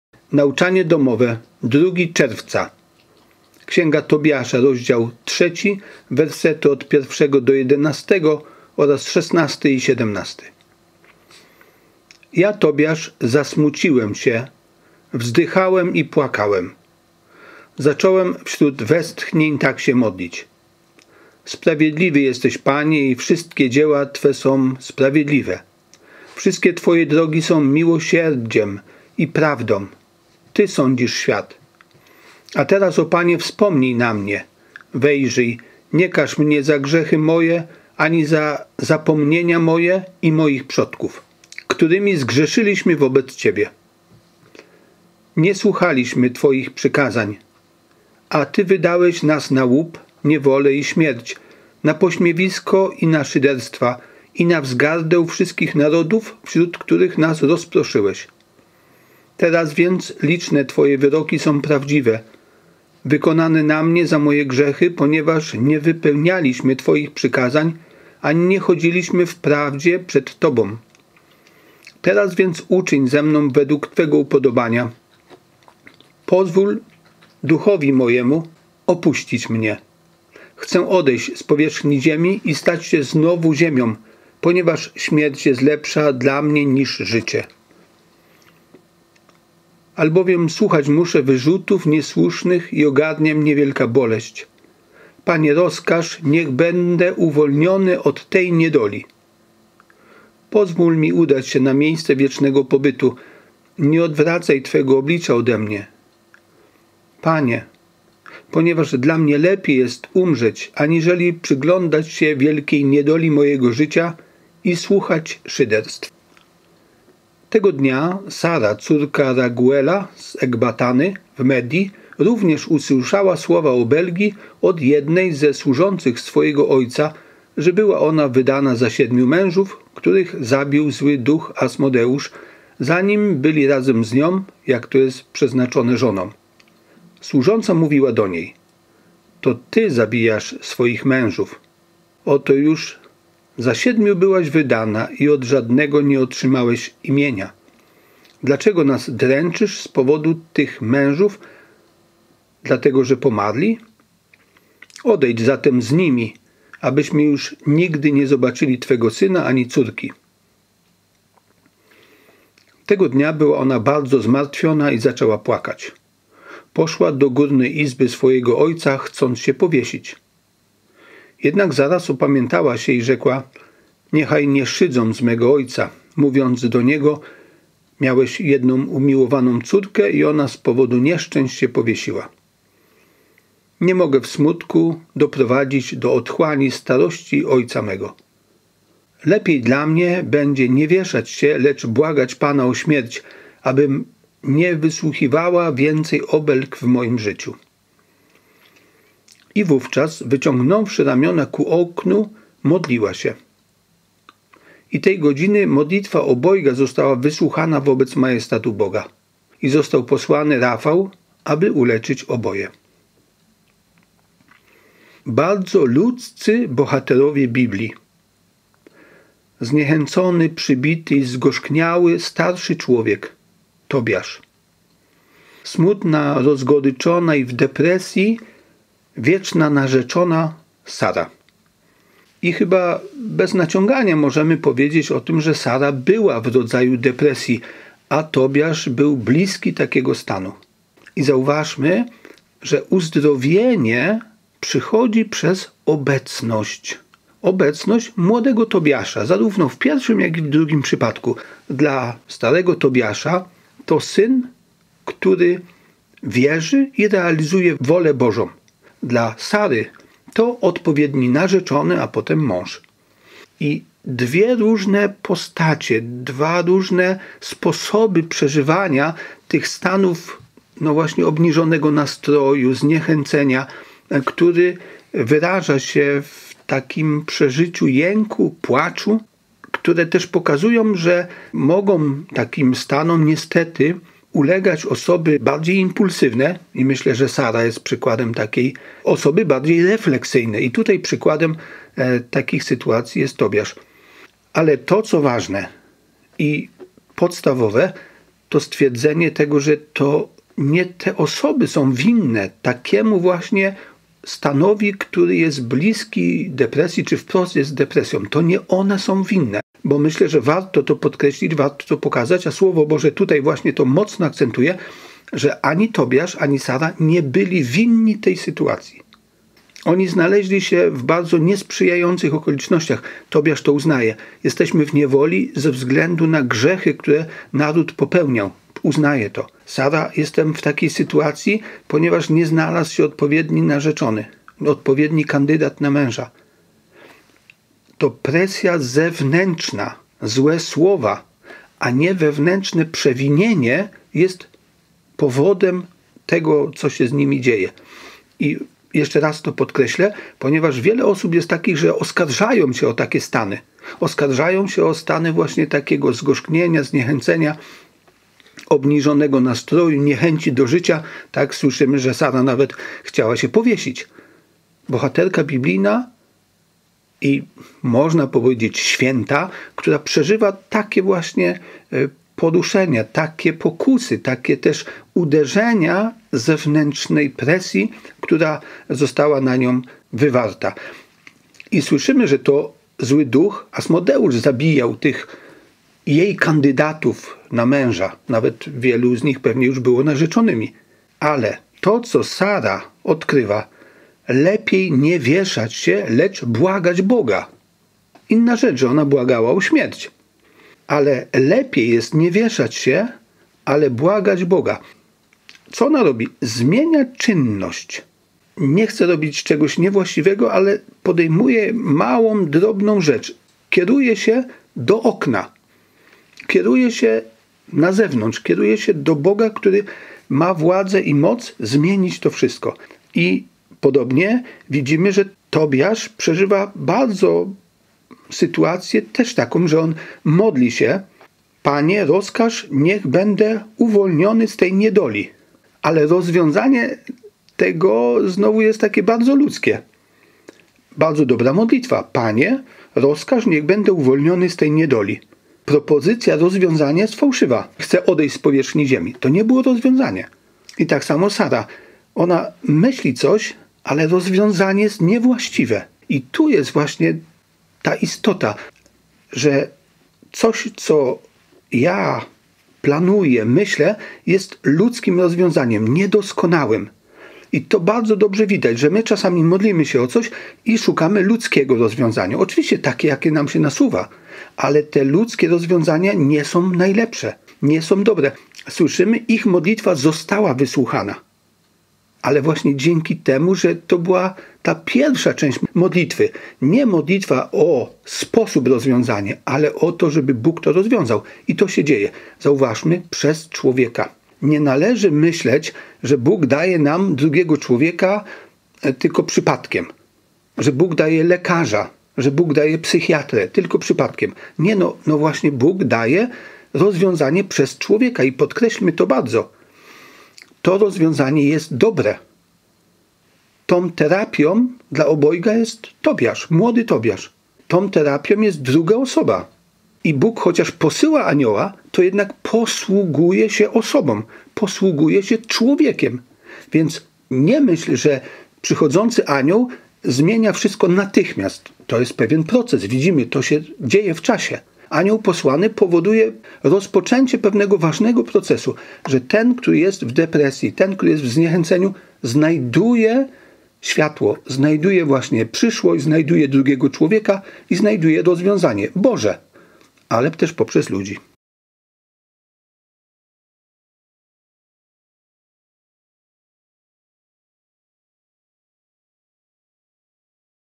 NauczaniaDomowe – L1#60 - Gorycz i depresja - ArturSepioło – 02.06.2021
Dwoje ludzi w krytycznej, depresyjnej sytuacji modli się o uwolnienie. Posłany przez Boga anioł uzdrawia oboje poprzez ludzi, którzy się znaleźli na drodze ich życia. Obecność ich jest lekarstwem.

Informacje ogólne:
Prowadzący:
Kamera:
Grafika kanału: Robert Mierzwiński